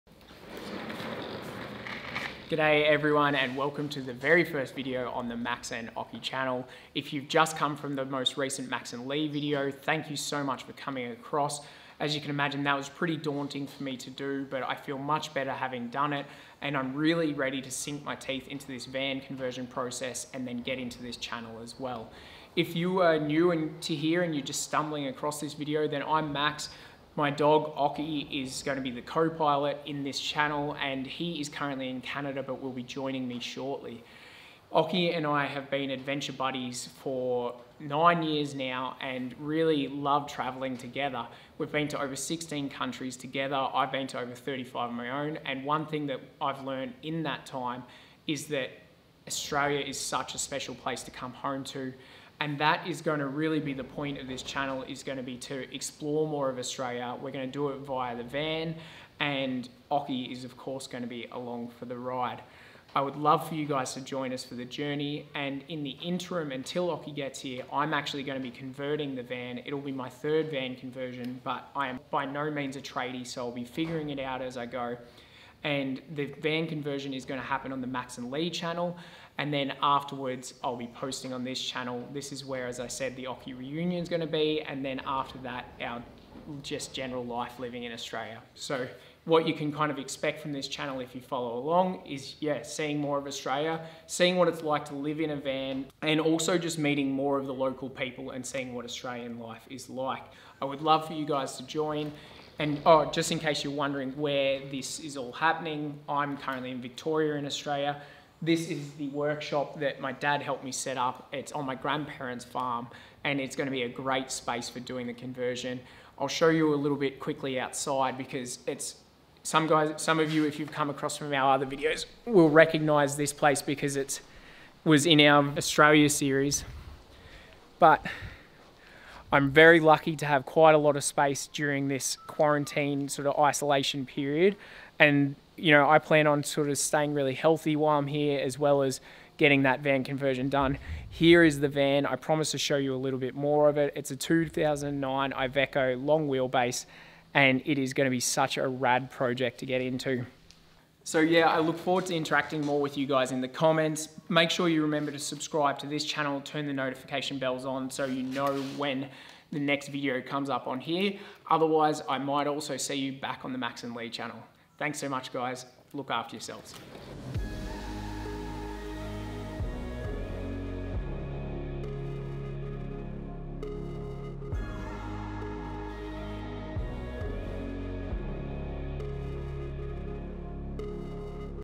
Max & Occy | A New Beginning | Van Life Australia
I'm Max & the furry (*furrier) one is Occy. He's an Aussie Shepherd and my best mate. We like to adventure together and our preferred method is by road. Or by van on the road to be exact. After recently finishing a journey that took 2 years and had us overlanding from Canada to the base of South America (15 countries in total) in our DIY camper, we have decided it's time to head home. This new channel is here to document and share our adventures as we live and explore Australia in our soon to be completed new van.

We would love for you to join us & share the highs and lows, epic landscapes, and meet the locals as we set out to explore more of this wonderful country.

Hope to see you on the road!